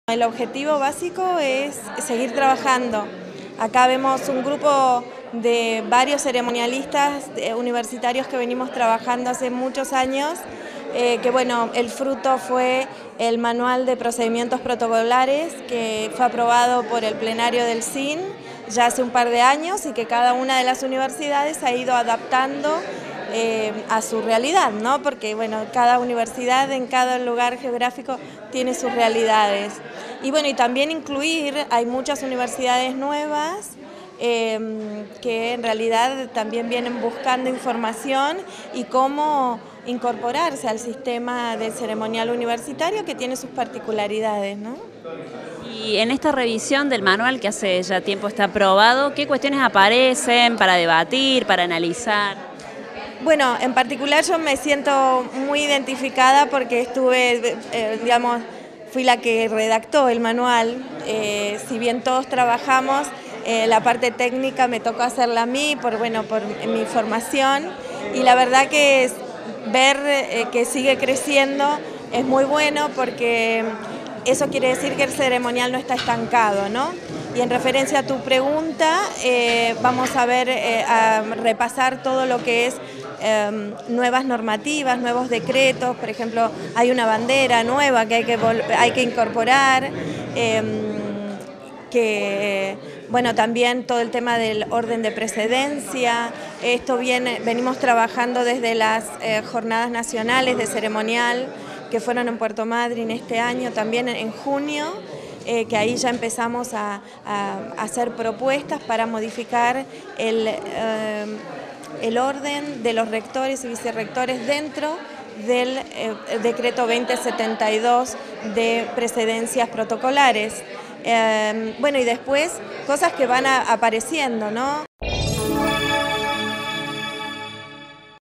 TALLER DE CEREMONIALISTAS UNIVERSITARIOS - KARINA MC CASKILL – UNIVERSIDAD NACIONAL DE RÍO NEGRO
DESTACAN EL ESPACIO DE INTERCAMBIO PARA LA GENERACIÓN DE UNA RED DE TRABAJO
Facebook.com/uniriotv
Twitter.com/uniriotv